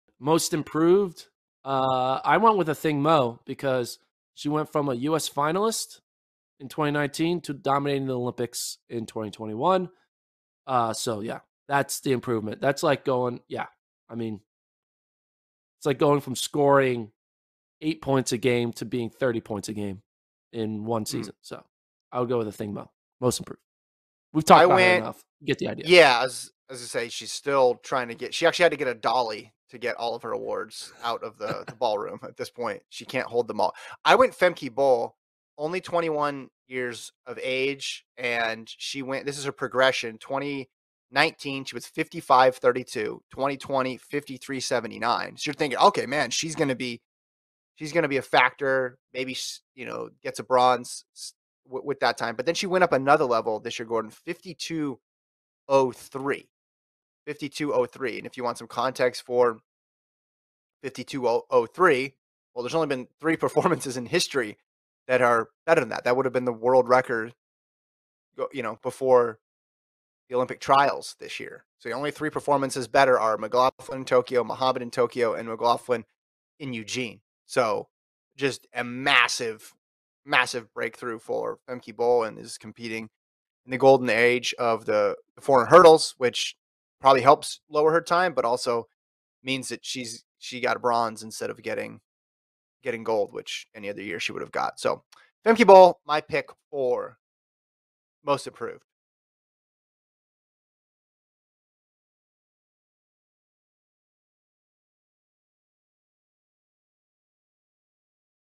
2021 Most Improved Athlete of the Year
Kevin and Gordon make their picks for most improved athletes of the year.